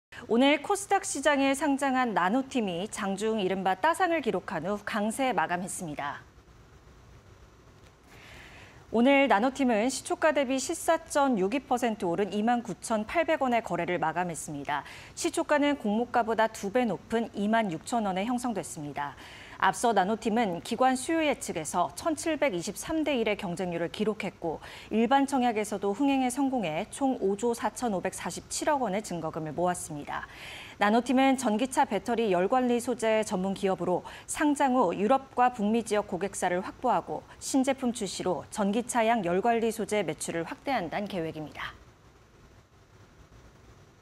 나노팀, 상장 첫날 '강세' 마감... 장중 '따상' 기록 (20230303)
나노팀, 상장 첫날 '강세' 마감... 장중 '따상' 기록